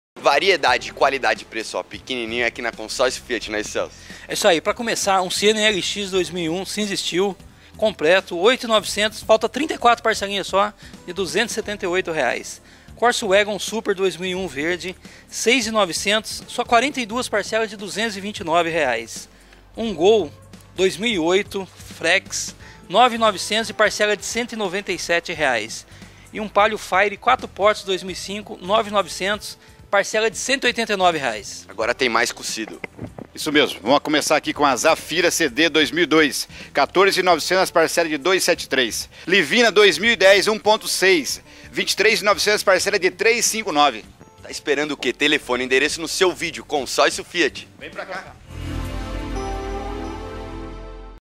Vale Shop - Programa 94 - Consórcio Fiat
Comercial produzido pela equipe Vale Shop e veiculado
no canal Band Vale, para o cliente Consórcio Fiat.